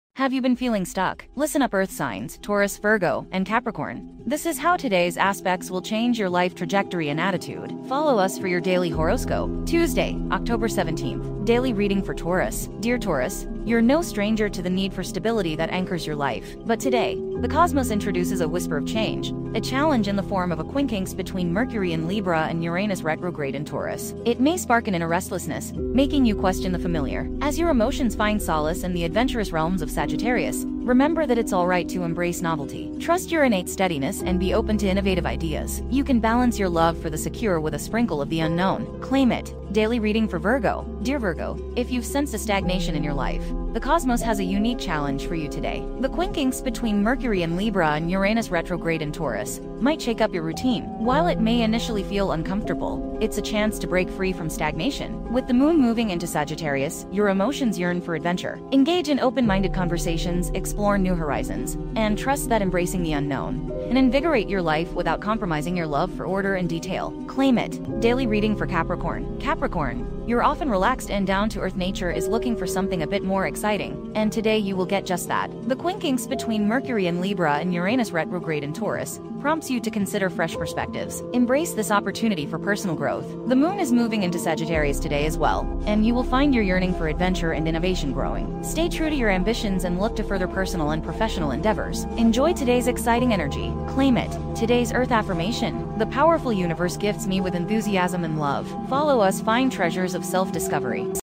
Daily Horoscope for Taurus, Virgo, and Capricorn - October 17th, 2023
Have you been feeling stuck? Listen up earth signs, Taurus, Virgo, and Capricorn, this is how today's aspects will change your life trajectory and attitude. Follow us for your daily horoscope.

Welcome to Elemental Star Reads!
Daily Fire Signs Horoscope for Taurus, Virgo, and Capricorn
Daily Horoscope reading for October 17th, 2023

Today's Earth affirmation:
The powerful universe gifts me with enthusiasm and love.
Follow us find treasures of self-discovery.